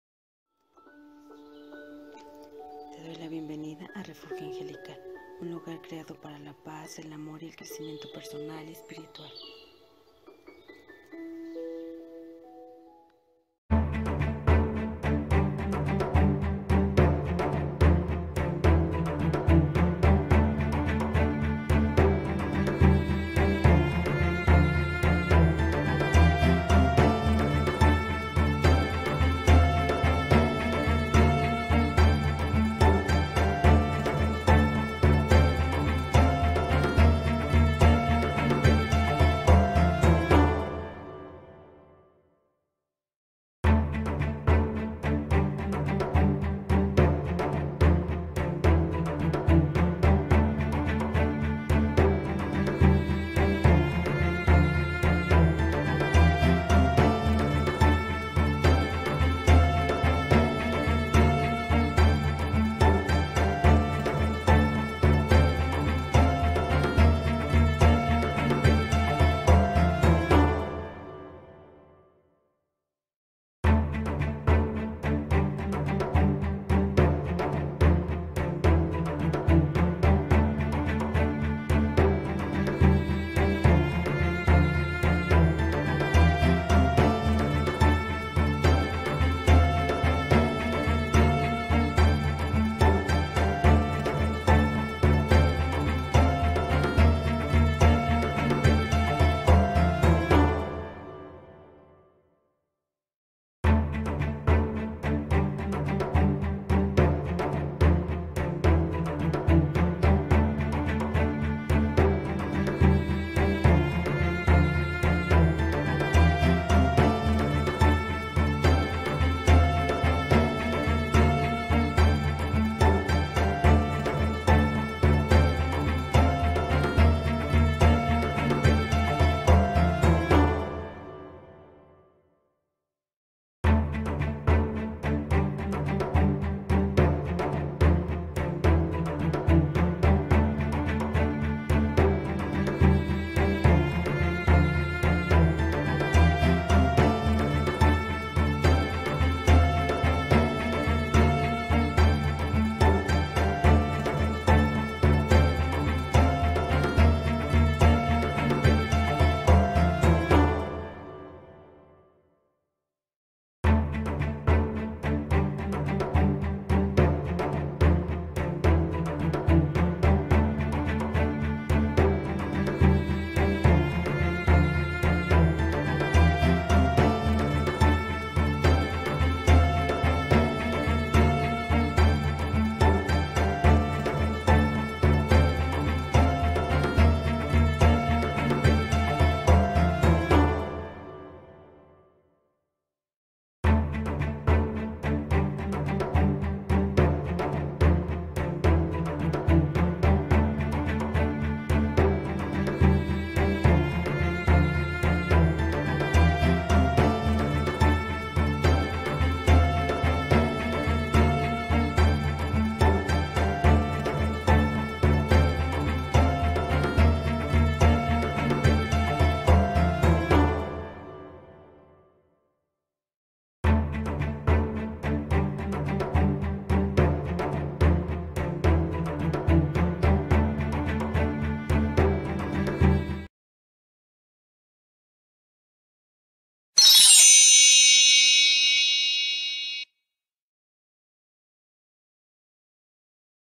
Mensaje de Tus Ángeles MIEDO
Por que te toco escuchar esto. Será que te paraliza el MIEDO, es hora de continuar e ir Mucho mas …allá Nunca Dejes de Soñar🌈💕✨